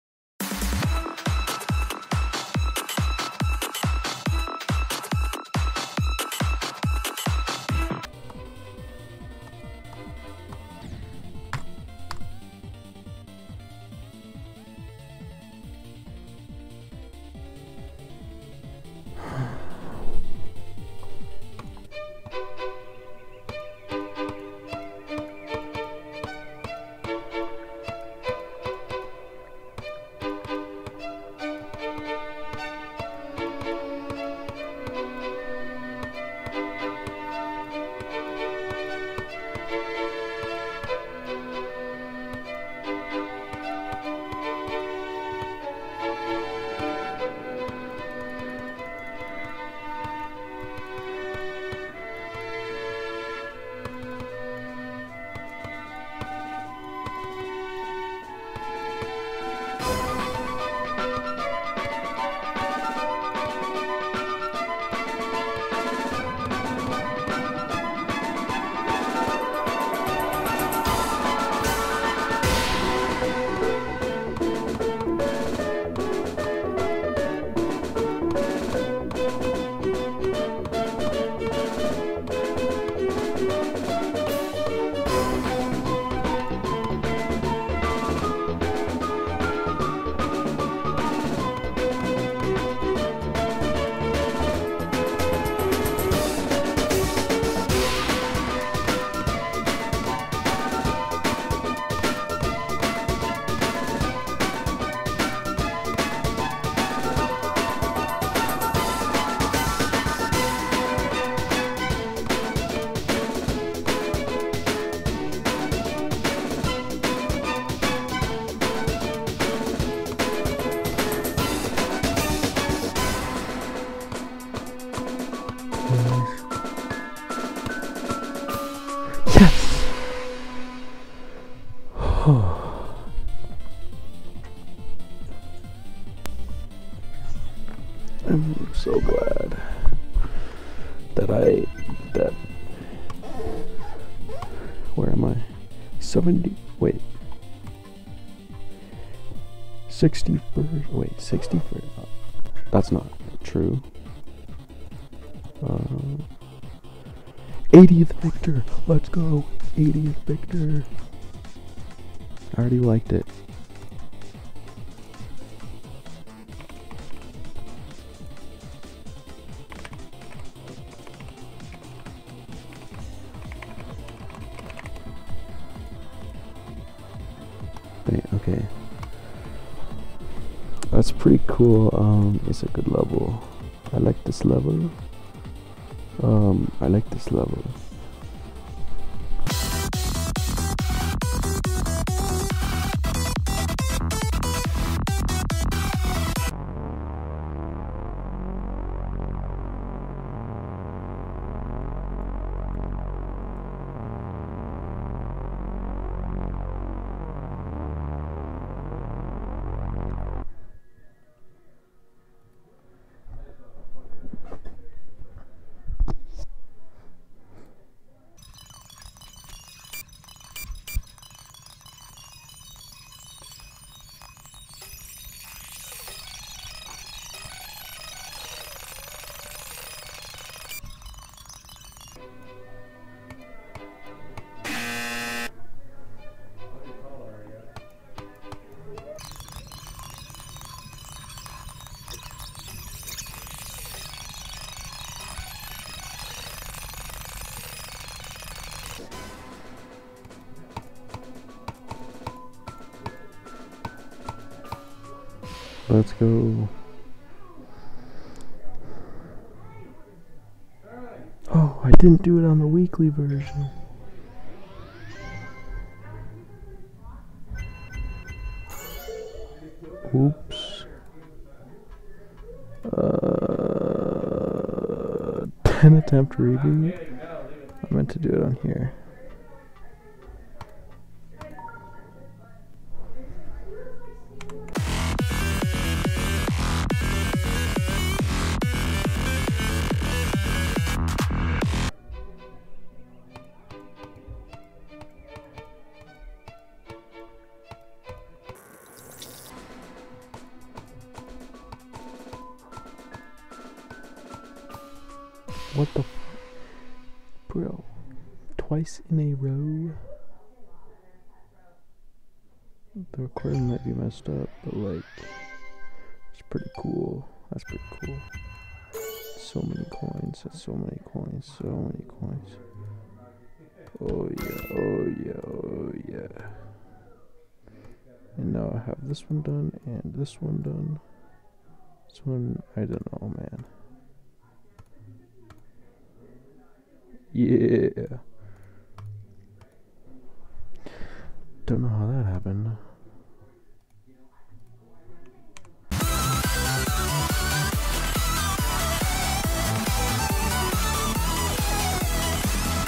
Demon #52 & 53 - Glorious Fortress
273 Attempts
1st rebeat: 37 attempts
2nd rebeat: 1 attempt
Total: 311 attempts
(Combining the 27 on the weekly ver. because that was done before the 1st rebeat)

This is a good level for sure
I feel bad for rebeating a weekly level but to be honest whatever I beat it three times